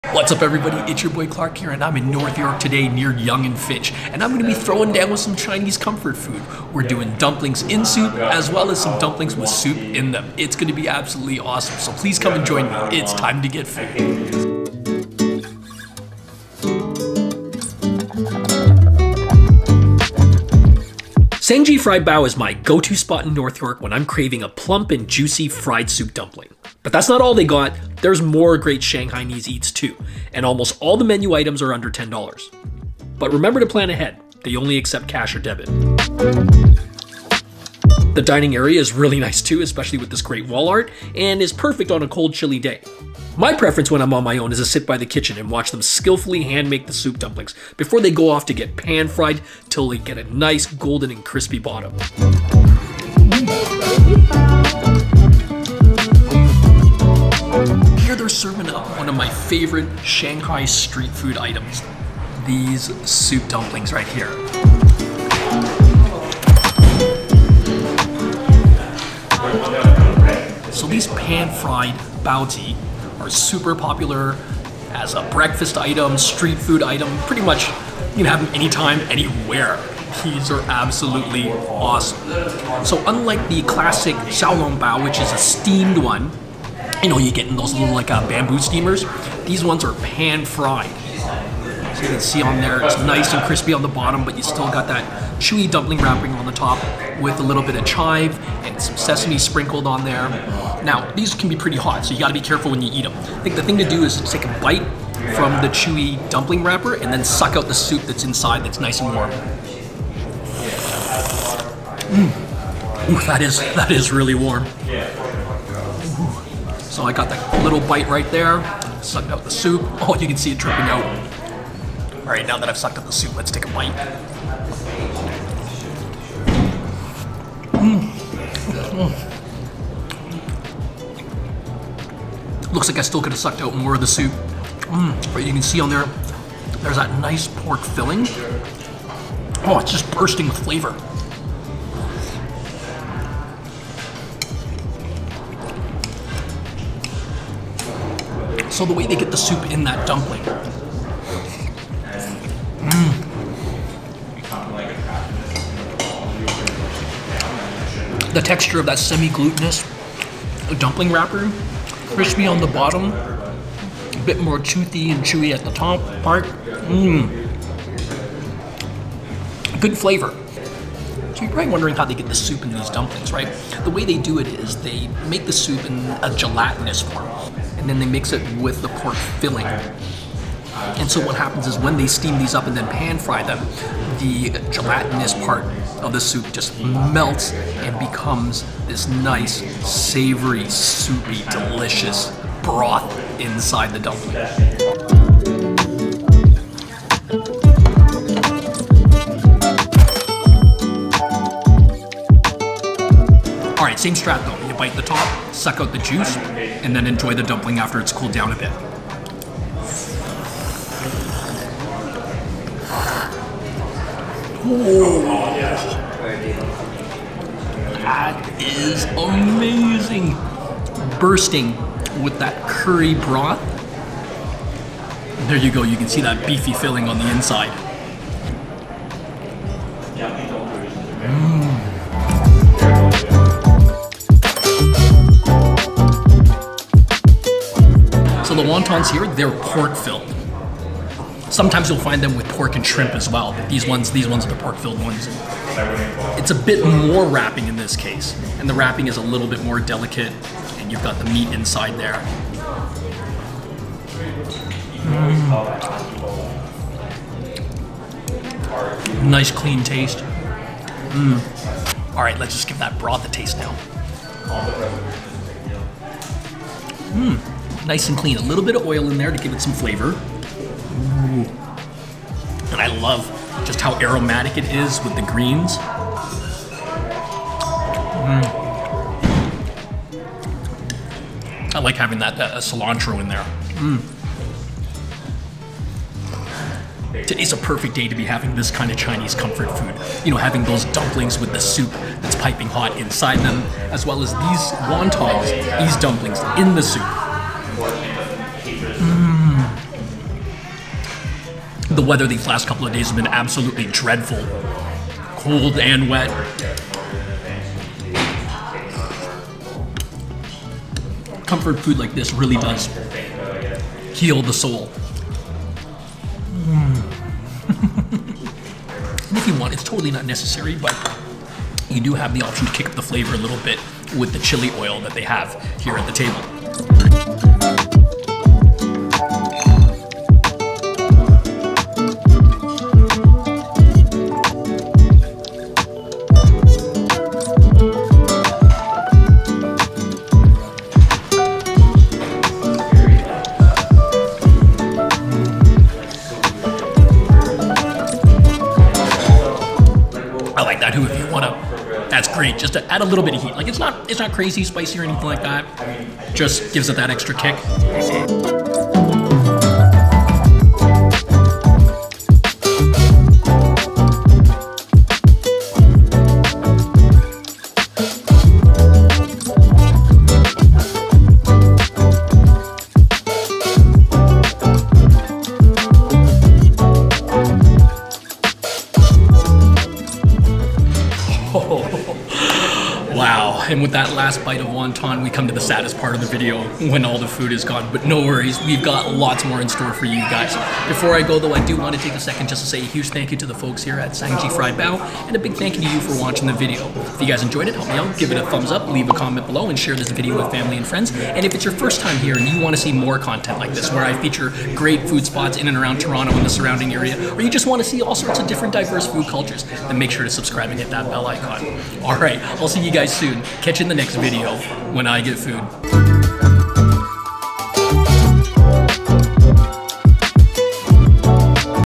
I BURNED My Tongue On These SOUP FILLED DUMPLINGS And It Was Worth It - Sang-Ji Fried Bao
Cold weather, get some soup dumplings or dumplings in soup. Specifically, get Sang-Ji Fried Bao. See for yourself, but just be careful and don't get too excited and slurp too fast - it's time to get food.

00:00 - Intro
00:58 - 2 kinds of Soup Filled Fried Bao
03:54 - Bite size Wonton in Soup